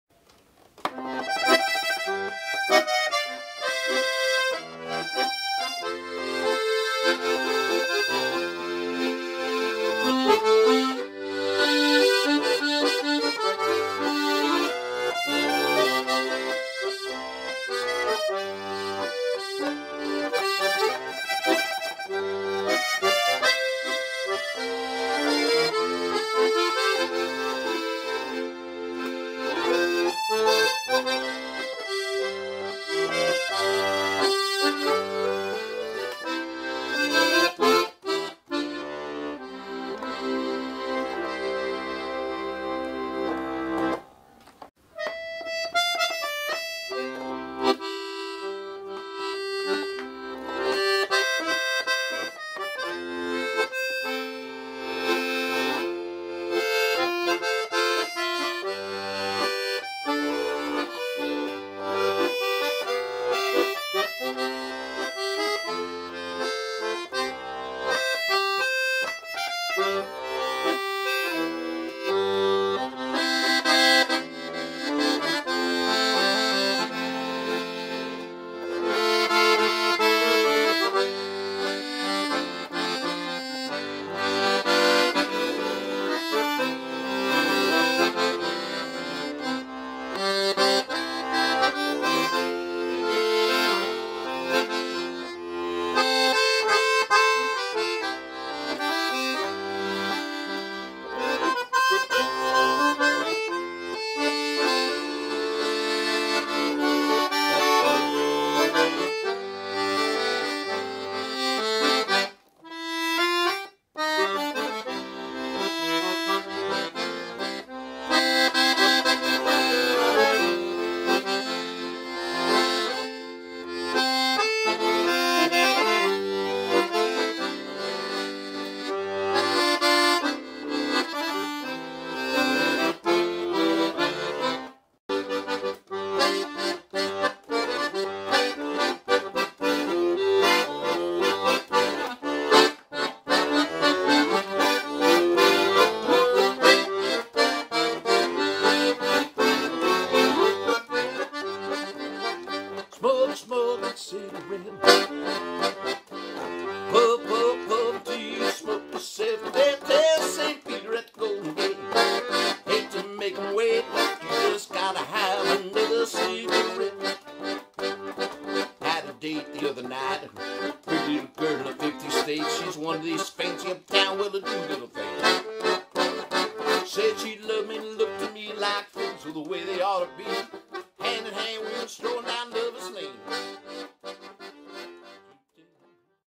Old Accordion Demo
90 Year old accordion demo. Owned by my father. Some songs he would have played on it.